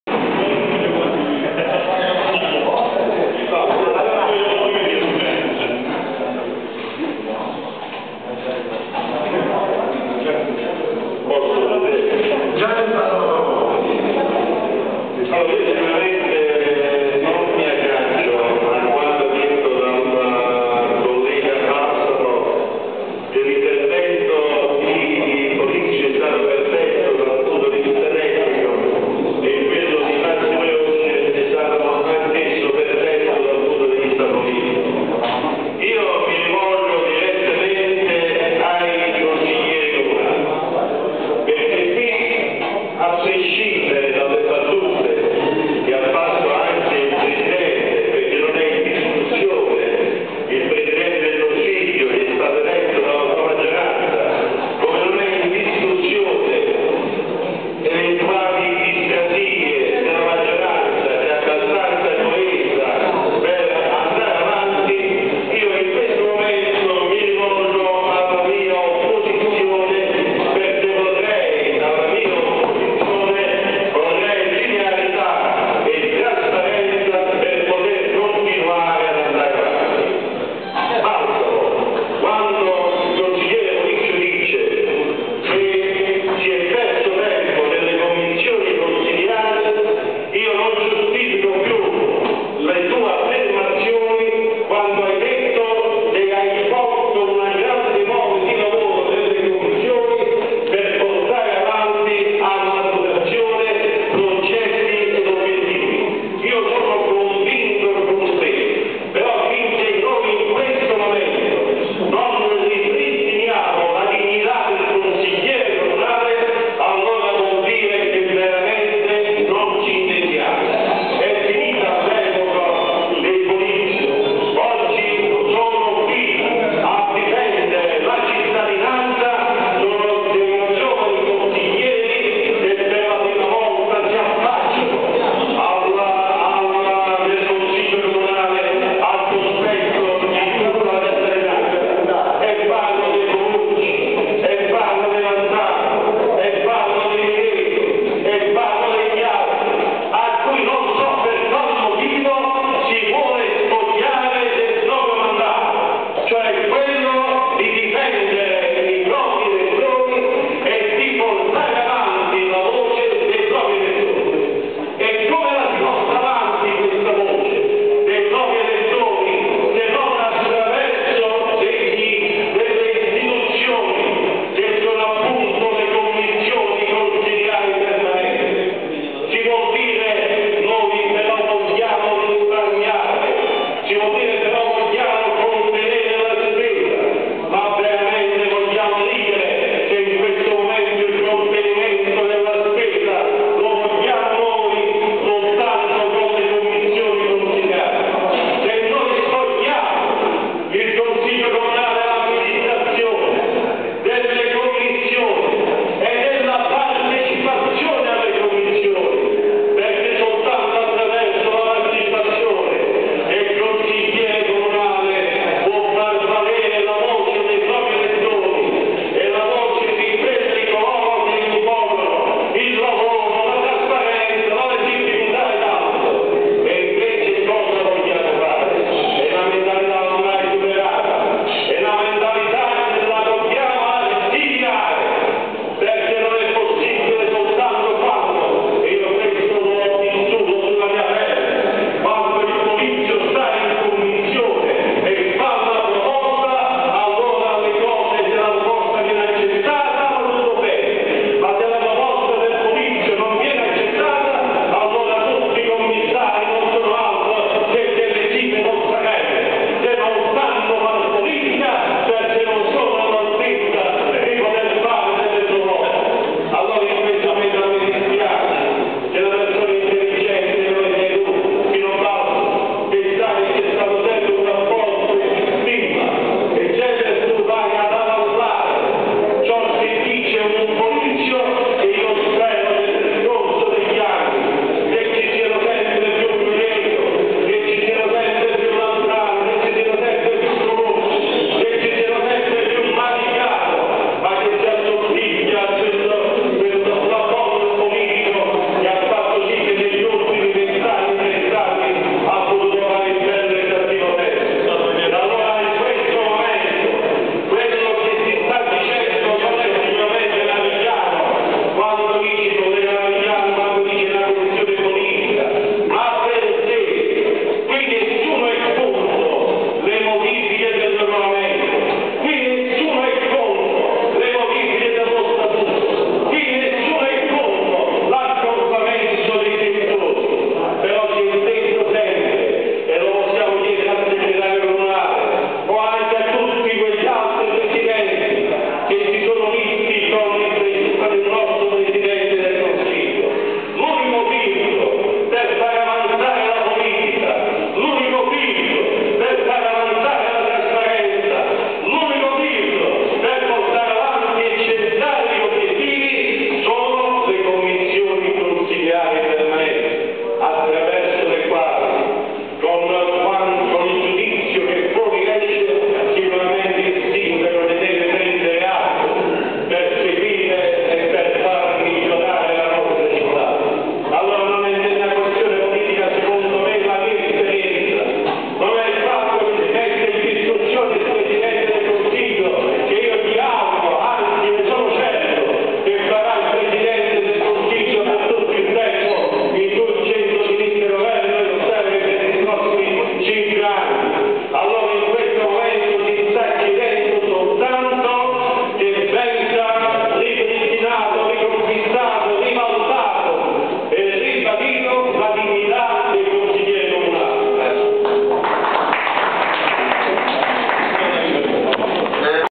CASORIA 20/10/2011 - Consiglio Comunale (25)
"Casoria Ieri & Oggi online" presenta:
Casoria giovedì 20 ottobre 2011,  ore 12.00, presso la Sala Consiliare, sita in piazza Cirillo, seduta del Consiglio Comunale.
All'ordine del giorno:
Approvazione verbali sedute del: 19, 21, 26 e 28 settembre 2011;
Modifica del Regolamento del Consiglio Comunale;
Istituzione della Commissione Consiliare Permanente, ex. art. 19 del vigente Regolamento.
Intervento del consigliere Stefano Ferrara (Pdl)